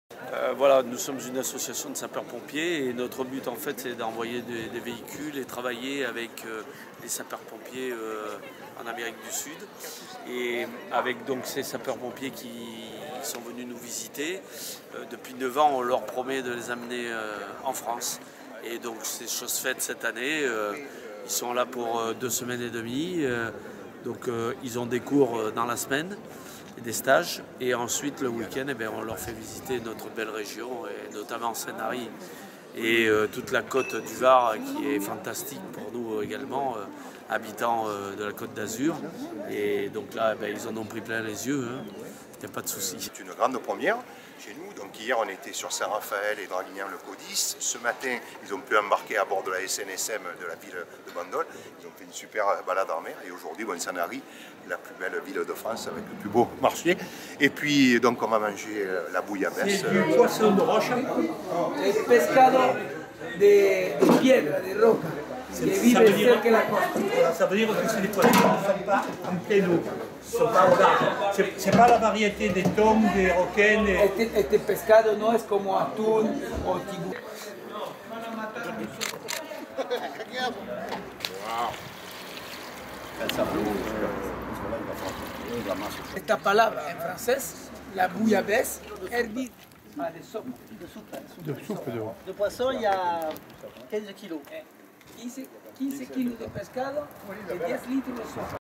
Pompiers d'Argentine à Sanary
Dimanche, José Tumméo (Président Sapeurs Pompiers Actions Internationales) a fait visiter Sanary à des pompiers d’Argentine menés par le commandant de la Caserne de Lujan de Cuyo, Tiritera Gustavo. Car ces pompiers argentins suivent des manœuvres et formations en semaine et profitent du week-end pour découvrir la Région. Avec Jean Luc Decitre (Président Union départementale des sapeurs pompiers du Var), les convives Argentins ont profité également d’une sortie en mer avec la SNSM de Bandol, et ont dégusté une délicieuse bouillabaisse au Centre de secours de Sanary concoctée par Danièle et Jean Brondi. Un beau moment d’échange et de partage.